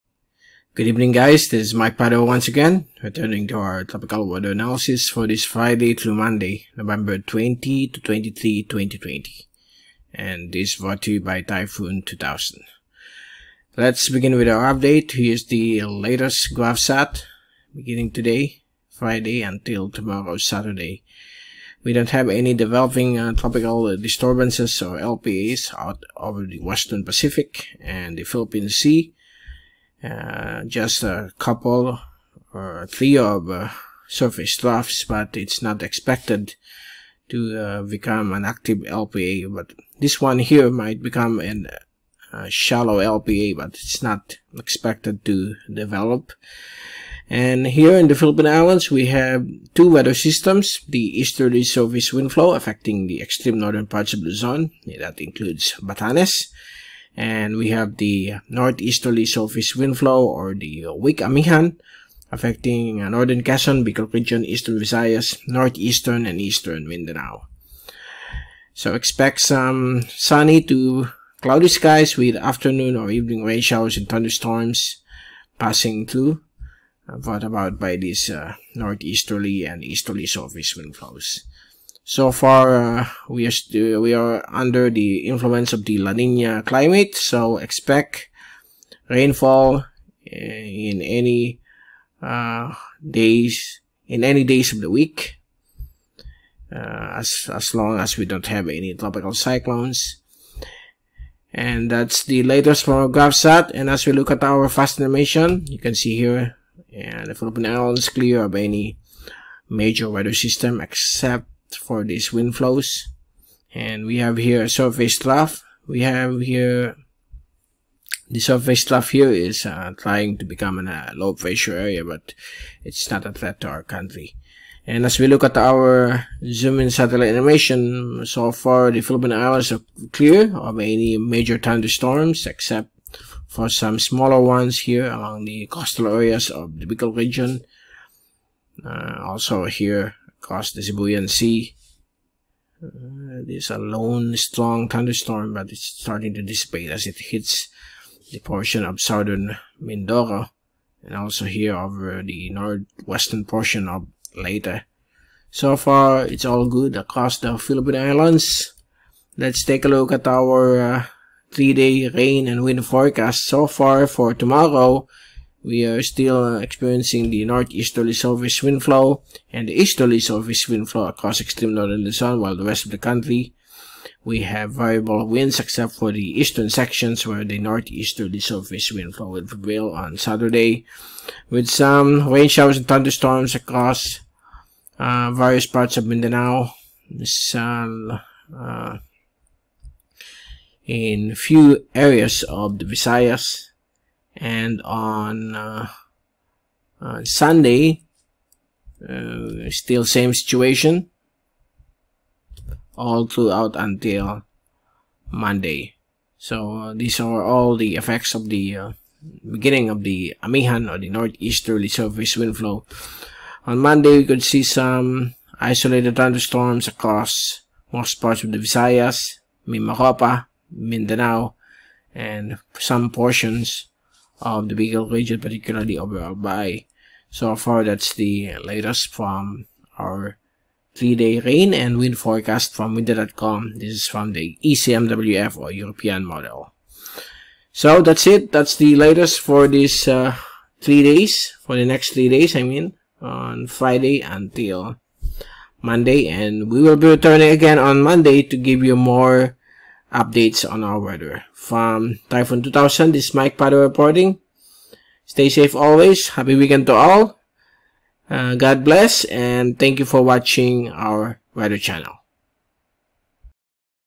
T2K Tropical Weather Analysis for Nov 20-23, 2020 (Fri-Mon) [English Ver]
No TC formation will be expected over the PH Sea & Western Pacific for the next 5-10 days.  Prevailing Northeasterlies will affect the eastern sections of the country.  To find out more, check out our T2K Tropical Weather Analysis recorded at 11:00 PM Manila Time (15:00 GMT) by Mike “Mr. Typhoon” Padua, Typhoon Specialist of Typhoon2000.com.